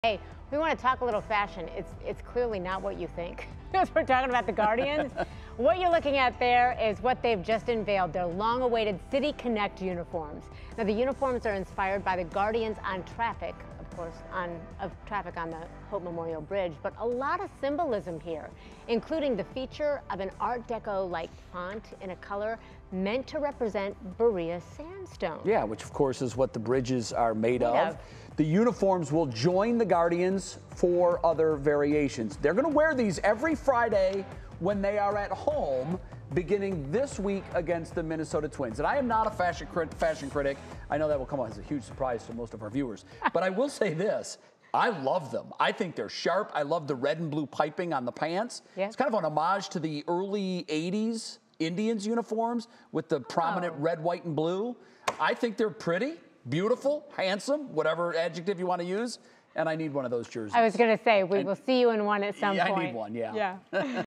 Cleveland Guardians unveil long-awaited City Connect uniforms
"We always rep the CLE."

Those were the words of rapper and Cleveland native Kid Cudi in a special video unveiling the Guardians special City Connect uniforms. The team finally made the long-awaited designs public Sunday evening, joining a trend that has become the norm across baseball since Nike took over manufacturing of the league's uniforms in 2021.

Heavily inspired by the famed "Guardians of Traffic" at the Hope Memorial Bridge outside Progressive Field, the navy blue uniforms feature the letters "CLE" emblazoned across the chest in an Art Deco-like font with a cream color meant to invoke Berea Sandstone. Red, white, and blue stripes make their way down the sides of the uniforms and pants, while the socks sporting images of the statues along with "EST. 1901," marking the first year of the ballclub's existence.

A red, white, and blue cap emblazoned with the "Diamond C" completes the ensemble. The team also unveiled what appears to be a new alternate logo spotlighting one of the Guardians gripping a baseball bat.